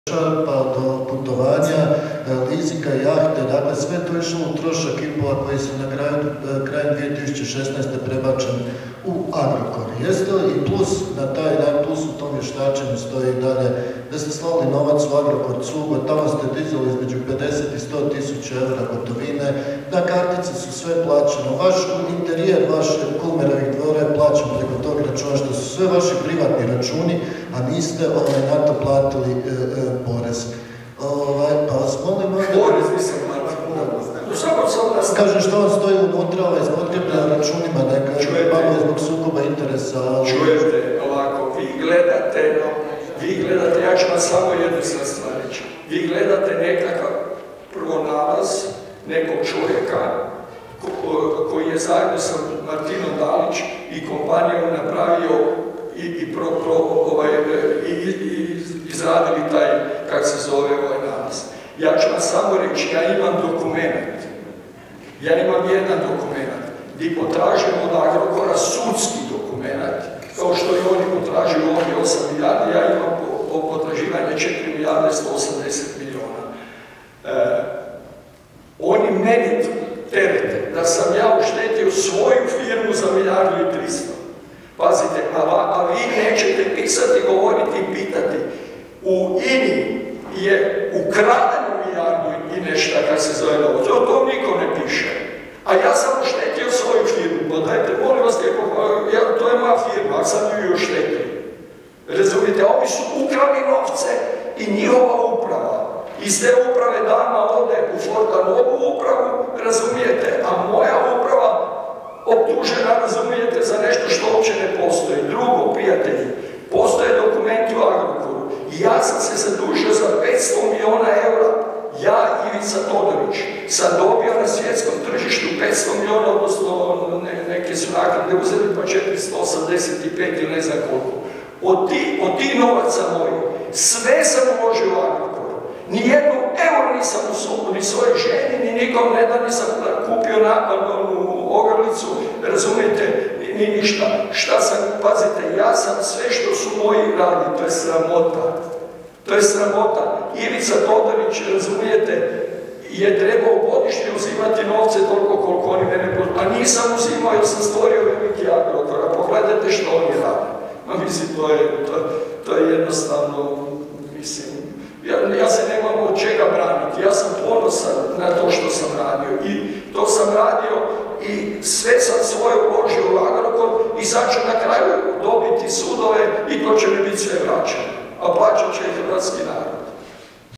Todorić: "Ja se nemam od čega braniti. Ponosan sam na to što sam radio"
Bivši vlasnik Agrokora Ivica Todorić održao je konferenciju za novinare. Teme koje je najavio su uloga izvanredne uprave Agrokora u kreiranju insolventnosti grupe Agrokor te financijsko- poslovno stanje Fortenove u odnosu na Agrokor.